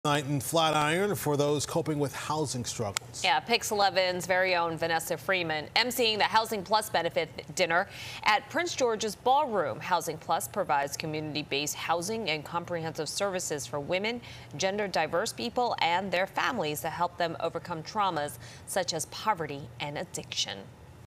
PIX11's Vanessa Freeman hosts Manhattan benefit dinner
PIX11's Vanessa Freeman hosted a HousingPlus benefit dinner focused on providing aid to families with housing struggles